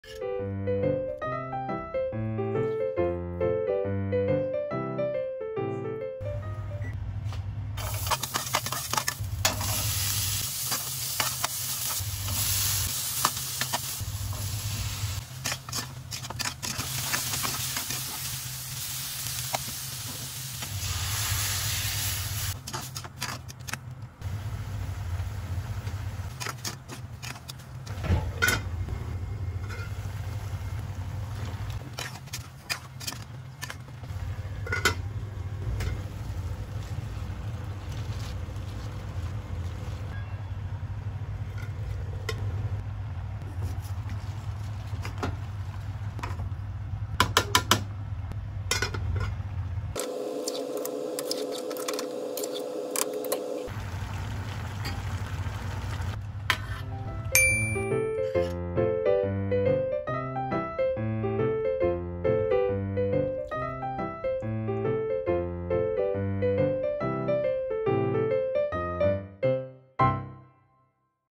Simple recipe , Ginisang Pechay
Ganito pag luto ko nang ginisang Pechay
Madalian 😊 pina kuluan ko na ang karne para  , tuloy tuloy na ang pag sangkutsa.